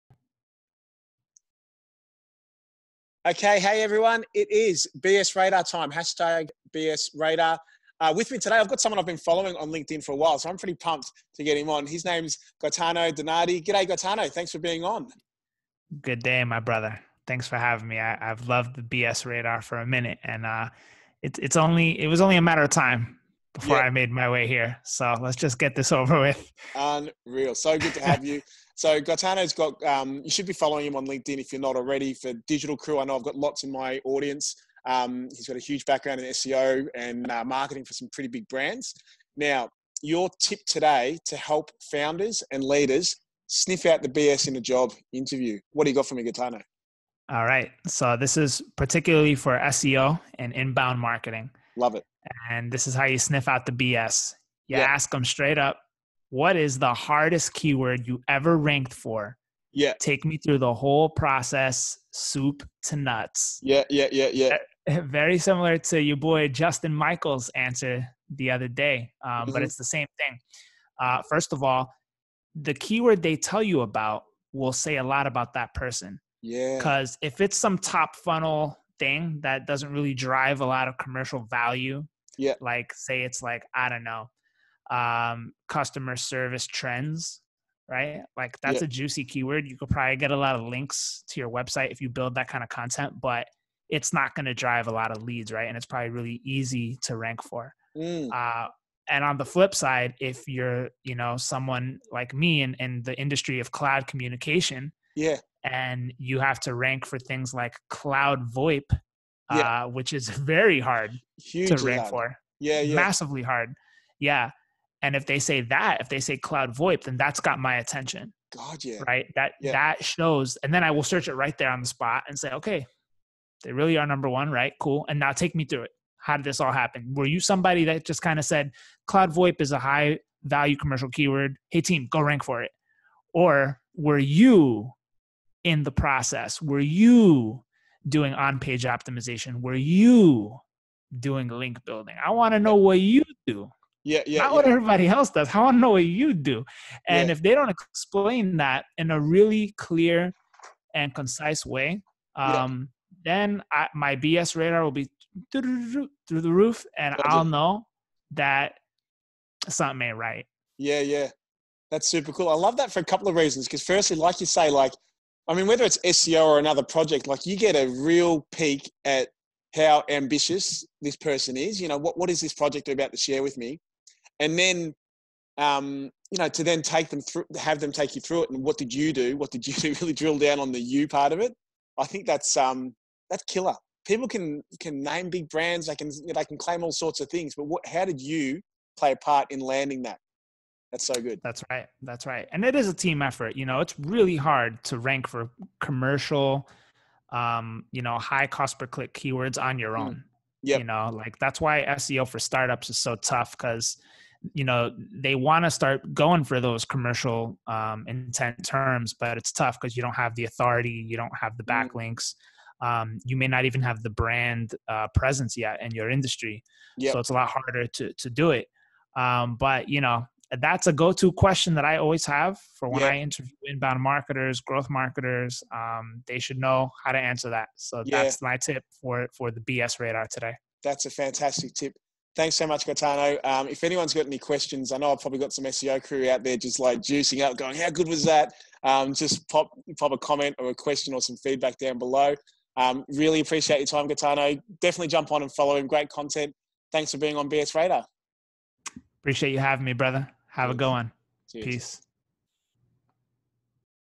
BSRadar x Gaetano Nino DiNardi
This week starring Gaetano Nino DiNardi, Director of Demand Generation at Nextiva.

Gaetano has built a formidable reputation in the SEO world: just this week was featured by NBC News' Today!

Over the years he has refined his BS radar through hundreds of interviews of would-be SEOs to work under his tutelage.

His number 1 hot tip to smell out the BS?

👃🏿 ASK THE CANDIDATE TO NAME THEIR TOUGHEST EVER PROJECT, AND WALK HIM THROUGH THE PROCESS FROM SOUP TO NUTS 👃🏿

The tip is specific to interviewing an SEO, but the way it paints a picture of the candidate is next level

▶️ You'll have to tune in to hear more ▶️

What do you reckon of Gaetano's bsradar?